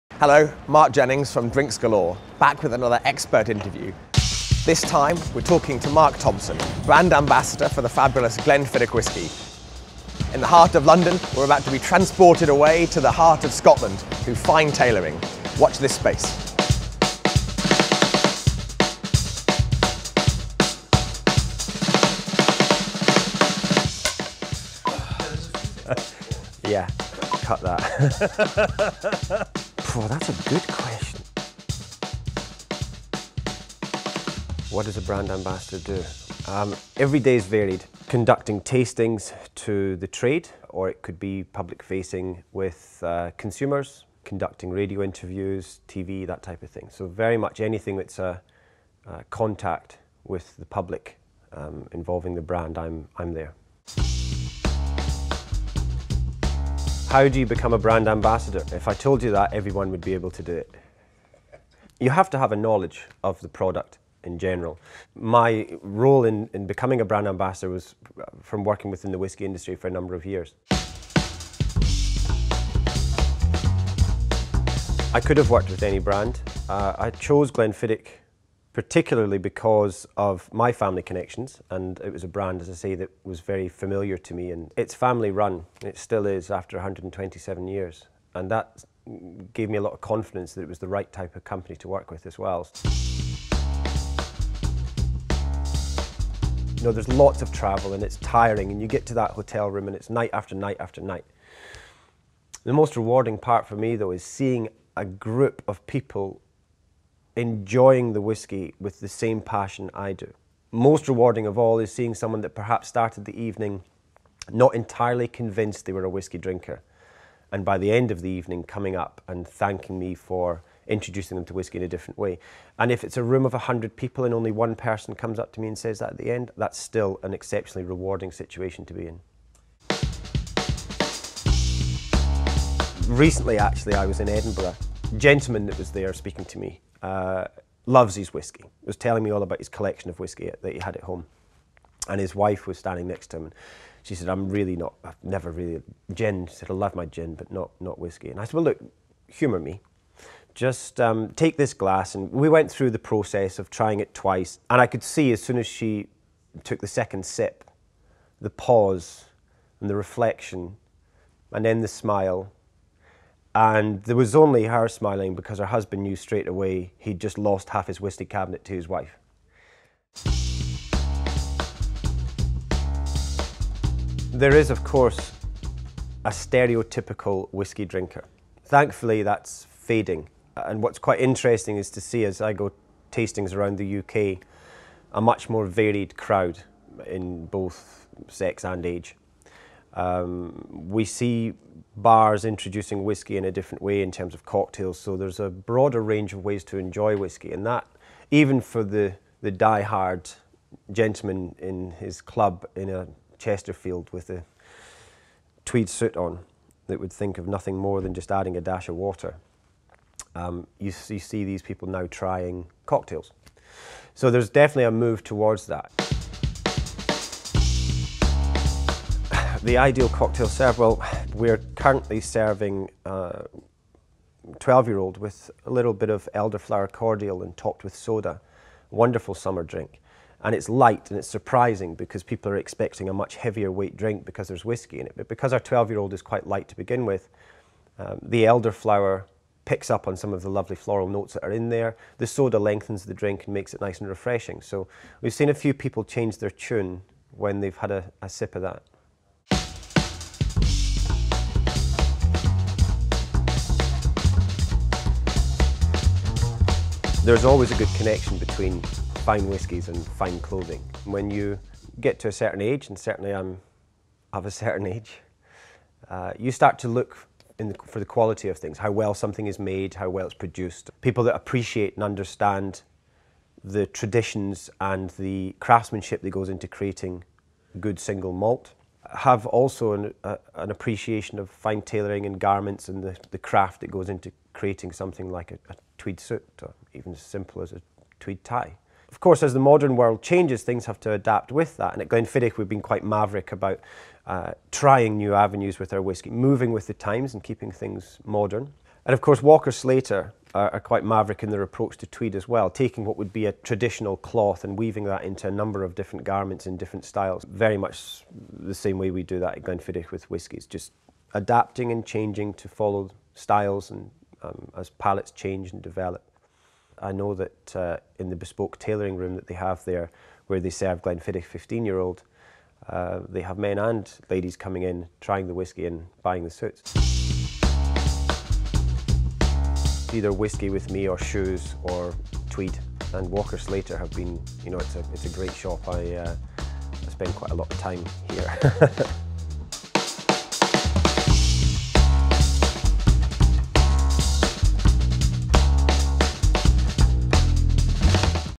Like whisky, have questions? Glenfiddich's ambassador Mark answers them
This short film answers a lot of whisky questions, with Glenfiddich's UK ambassador Mark Thomson

A Drinks Galore expert Interview

What does a brand ambassador do?
I don't like whisky, what about me?
Can I drink whisky in a cocktail?
Ever wondered what the link between fine whiskies and bespoke tailoring?

Featuring Mark Thomson, UK Glenfiddich Brand Ambassador
Thanks to Walker Slater Tailors  walkerslater.com